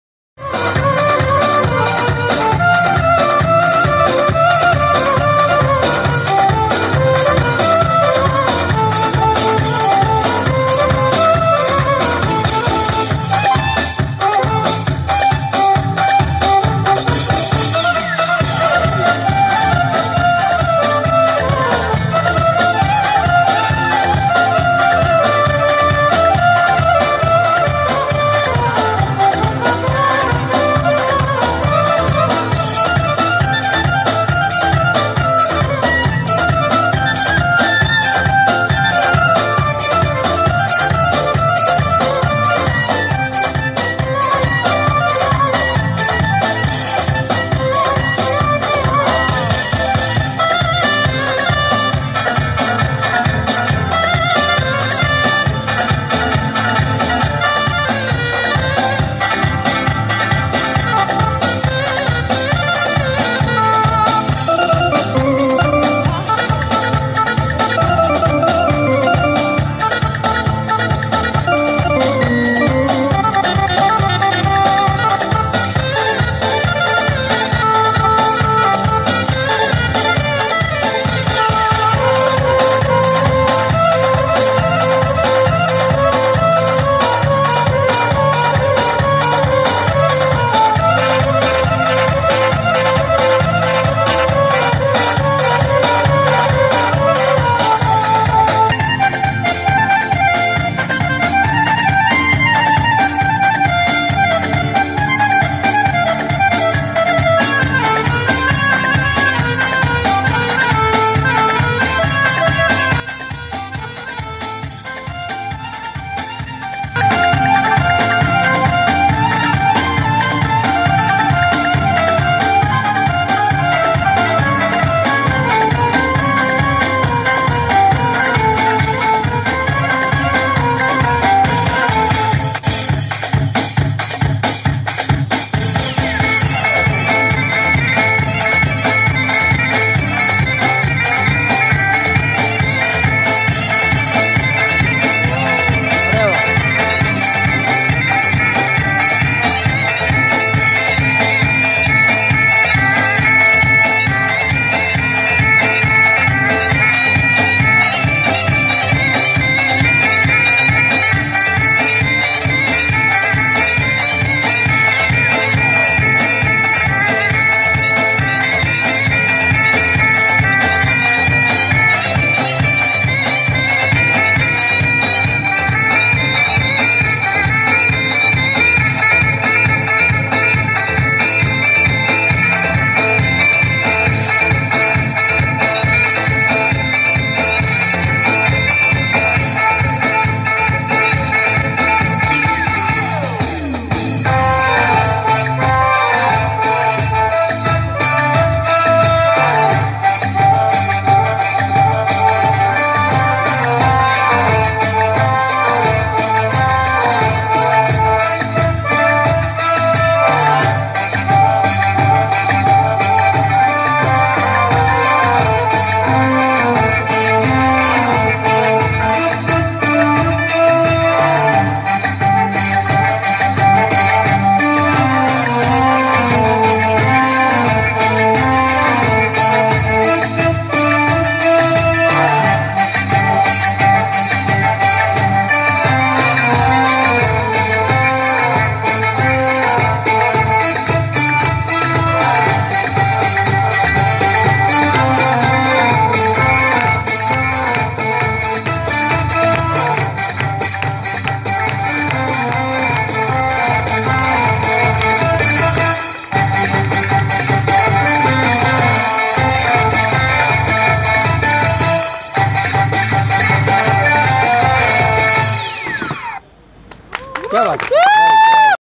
world record dance move at nsd
guinness world reocord dance move at national school of drama delhi 2017